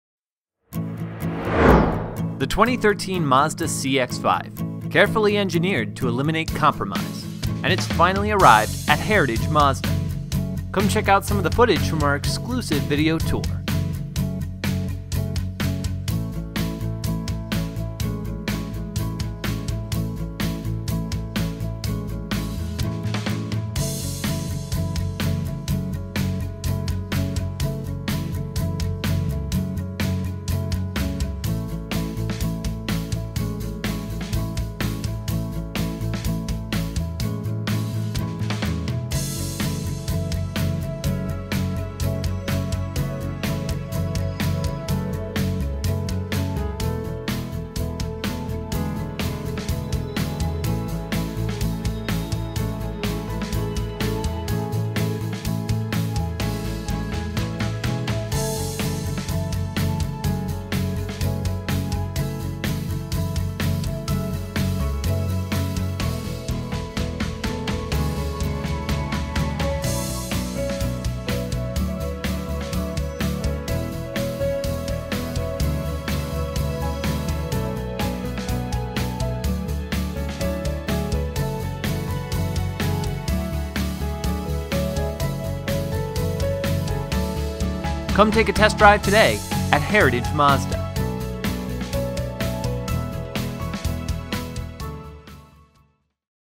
New 2013 Mazda CX-5 video overview at Towson MD Mazda dealer Baltimore
The new 2013 Mazda CX-5 has arrived to Heritage Mazda Towson in Towson, MD! Come check out the exclusive footage in our video tour.